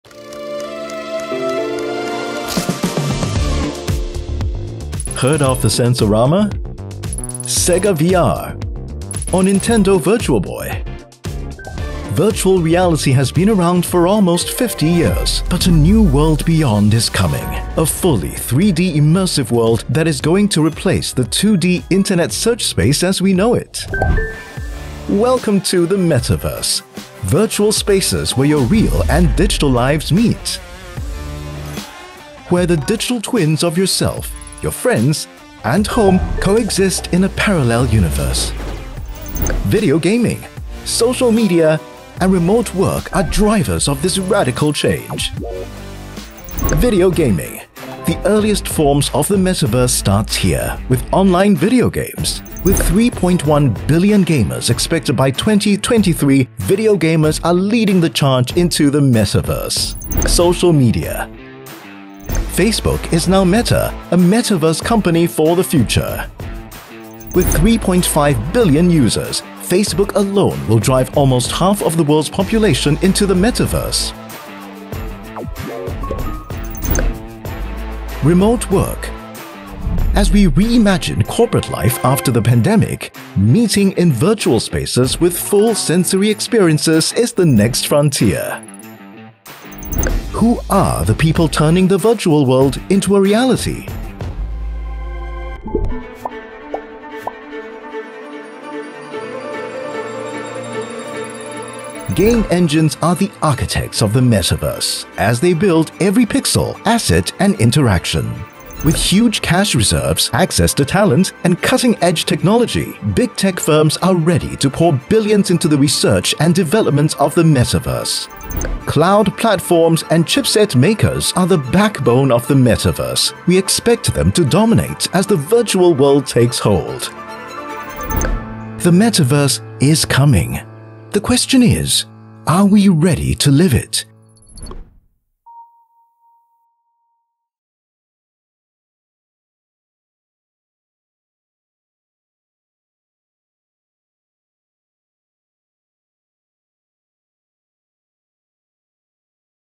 Discover the drivers and frontrunners of the Metaverse
The Metaverse, a world where our real and digital lives meet, will revolutionse the way we live, work, and play. Advertising, retailing, and remote work will never be the same again. We take a closer look at the drivers of this new world, and the companies that are turning the Metaverse into a reality.